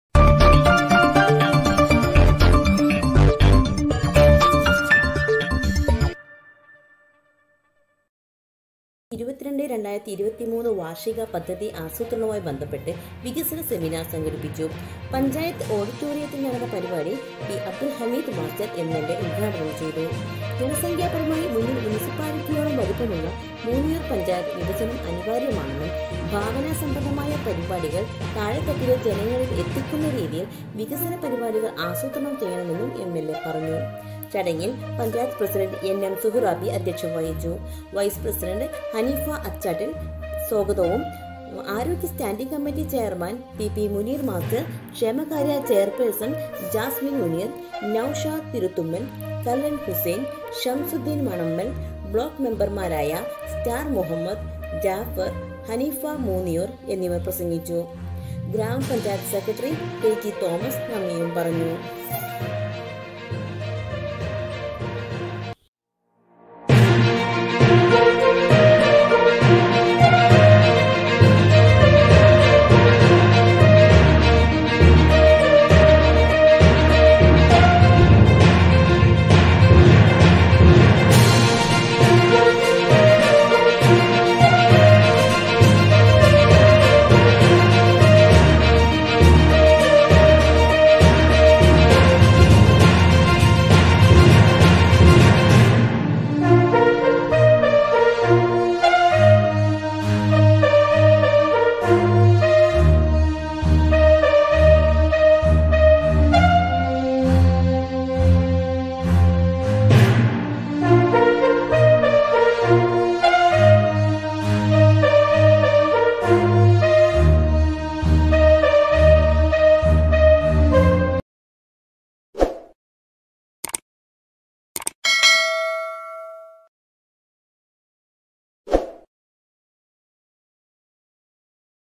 മൂന്നിയൂർ പഞ്ചായത്ത് വികസന സെമിനാർ സംഘടിപ്പിച്ചു.
മൂന്നിയൂർ ഗ്രാമപഞ്ചായത്ത് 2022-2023 വാർഷിക പദ്ധതി ആസൂത്രണവുമായി ബന്ധപ്പെട്ട് വികസന സെമിനാർ സംഘടപ്പിച്ചു.
പഞ്ചായത്ത് ഓഡിറ്റോറിയത്തിൽ നടന്ന പരിപാടി പി.അബ്ദുൽ ഹമീദ് മാസ്റ്റർ എം.എൽ.എ.ഉദ്‌ ഘാടനം ചെയ്തു.

ജനസംഖ്യാപരമായി മുന്നിൽ മുനിസിപ്പാലിറ്റിയോളം വലിപ്പമുള്ള മൂന്നിയൂർ പഞ്ചായത്ത് വിഭജനം അനിവാര്യമാണെന്നും ഭാവനാ സംബന്ധമായ പരിപാടികൾ താഴെ തട്ടിലെ ജനങ്ങളിൽ എത്തിക്കുന്ന രീതിയിൽ വികസനപരിപാടികൾ ആസൂത്രണം ചെയ്യണമെന്നും എം.എൽ.എ.പറഞ്ഞു.
ചടങ്ങിൽ പഞ്ചായത്ത് പ്രസിഡണ്ട് എൻ.എം.സുഹ്‌റാബി അദ്ധ്യക്ഷ്യം വഹിച്ചു.വൈസ് പ്രസിഡണ്ട് ഹനീഫ അച്ചാട്ടിൽ സ്വാഗതം പറഞ്ഞ്ഞു.ആരോഗ്യ സ്റ്റാന്റിംഗ് കമ്മറ്റി ചെയർമാൻ പി.പി.മുനീർ മാസ്റ്റർ,കേഷമകാര്യ ചെയർപേഴ്സൺ ജാസ്മിൻ മുനീർ,നൗഷാദ് തിരുത്തുമ്മൽ,കല്ലൻ ഹുസൈൻ,ഷംസുദ്ധീൻ മണമ്മൽ,ബ്ലോക്ക് മെമ്പർമാരായ സ്റ്റാർ മുഹമ്മദ്,ജാഫർ,ഹനീഫ മൂന്നിയൂർ എന്നിവർ പ്രസംഗിച്ചു.ഗ്രാമപഞ്ചായത്ത് സെക്രട്ടറി ടി.ജി തോമസ് നന്ദി പറഞ്ഞു.